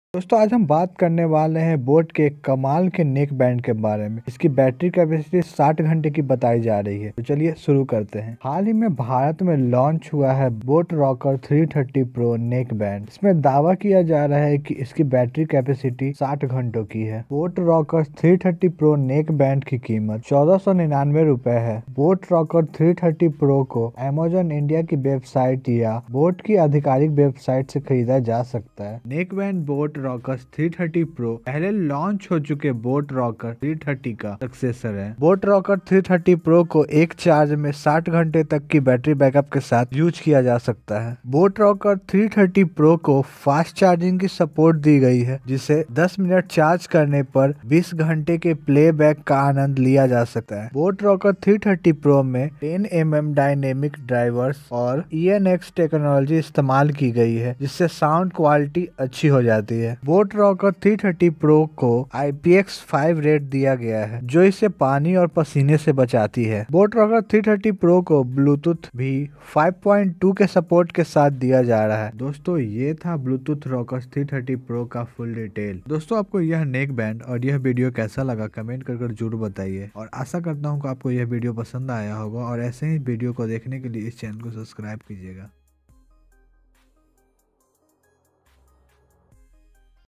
Boat Rockerz 330 Pro neckband all details#boatRockerz330Pro#Boat#i4you#i4u
Brand             boAt
Model             Rockerz 330 Pro
Type              In the Ear
Connectivity      Wireless

DESIGN AND BODY

Foldable।                  Yes

FEATURES

Bluetooth                  Yes, 5.2
Bluetooth Range।     10 m
Microphone।           Yes
Water Resistant           Yes
Switch between
 Call and Music।          Yes

Additional Features

ASAP Charge (10 minutes = 20 hours), IPX5 Rated

SOUND FEATURES

Deep Bass                  Yes
Frequency Response  20 Hz (Min) - 20 KHz (Max)
Driver Unit।             10 mm
Driver Type।            Dynamic
Other Features          ENx Technology, boAt Signature Sound

EXTRA

Sales Package।
                           1 Headphone, Additional            Eartips, Charging Cable, User Manual, Warranty Card

Inline Remote   Yes

POWER FEATURES

Battery Life         2.5 days

WARRANTY

Covered in Warranty Manufacturing Defects
Not Covered in Warranty Physical Damage